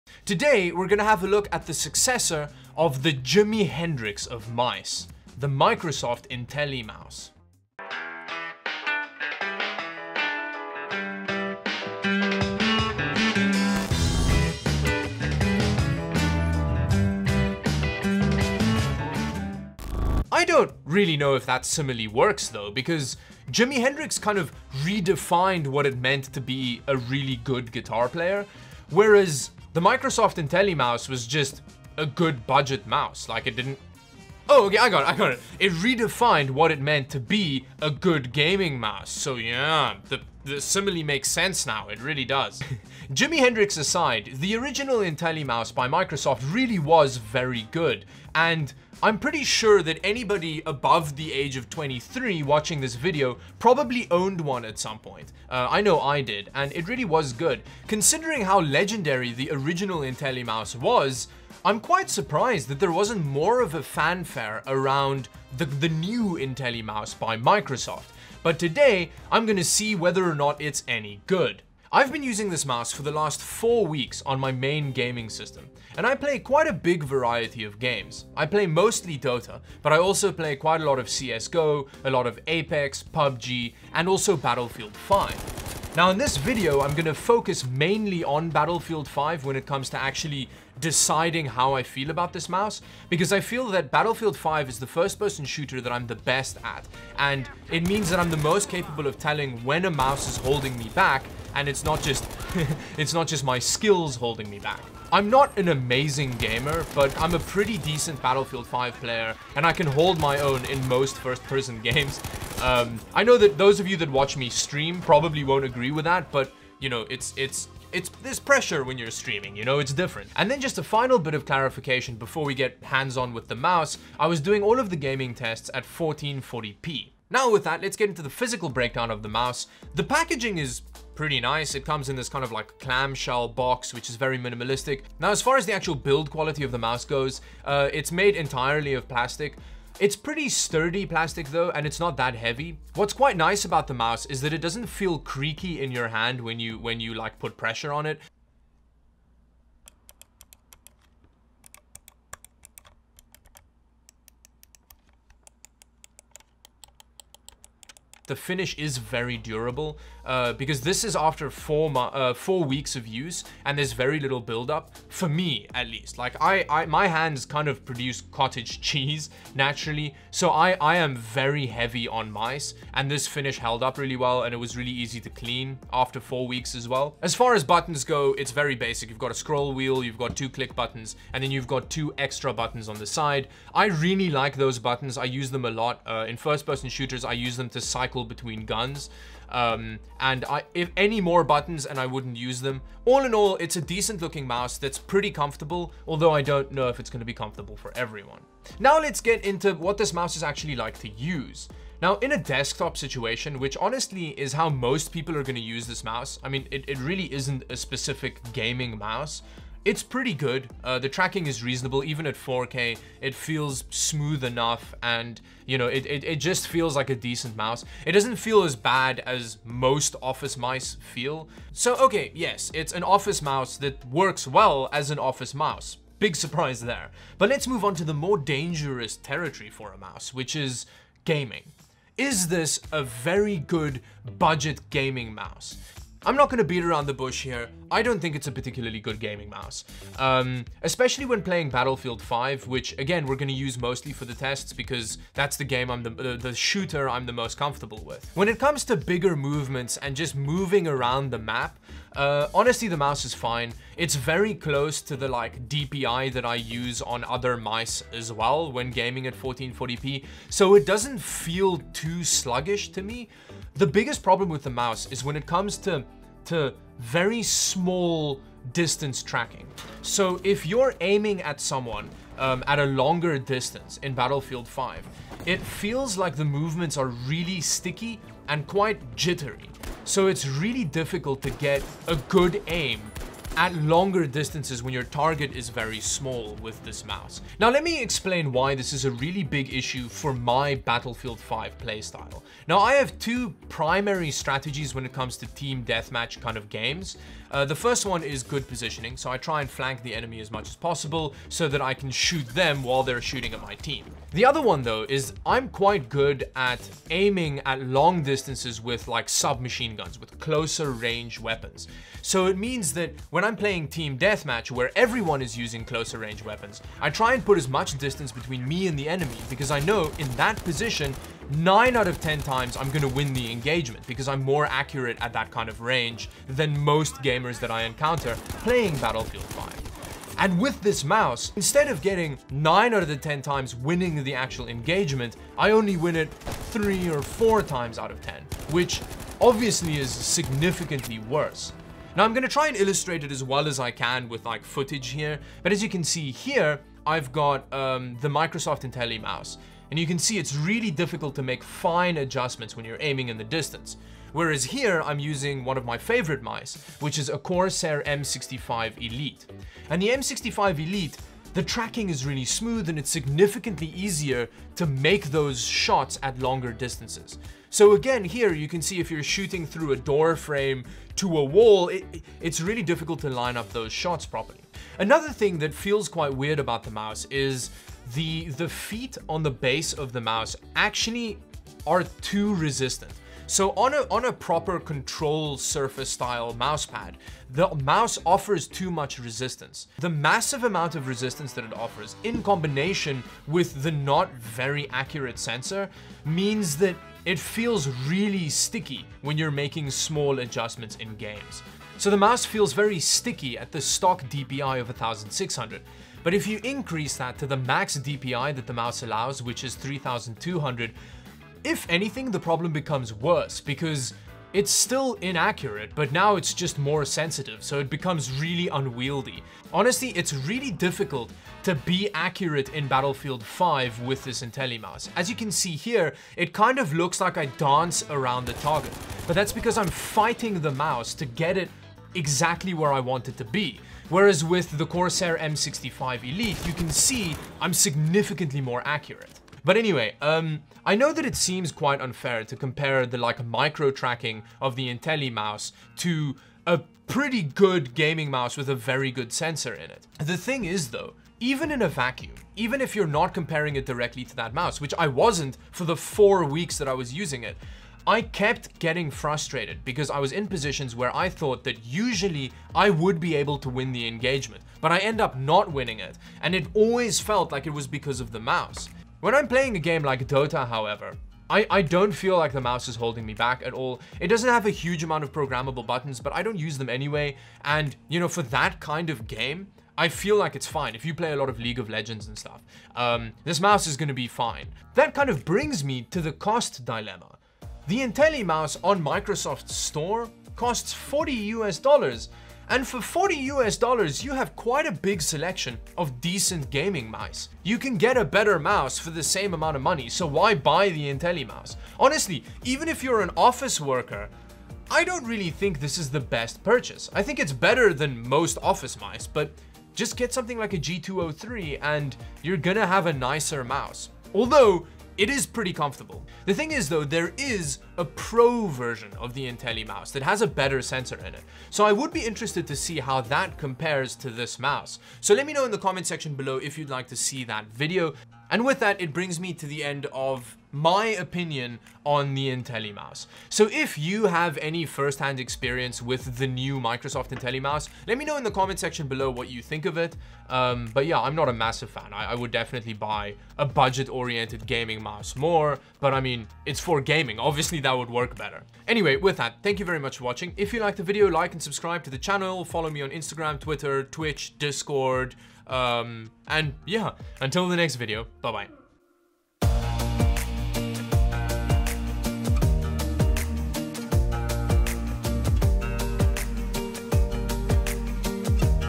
Is the new Microsoft Classic IntelliMouse a sleeper gaming mouse like the original?
The original Microsoft IntelliMouse was an amazing budget PC gaming option that many people swore by. Is the new Microsoft Classic IntelliMouse able to capture that same magic?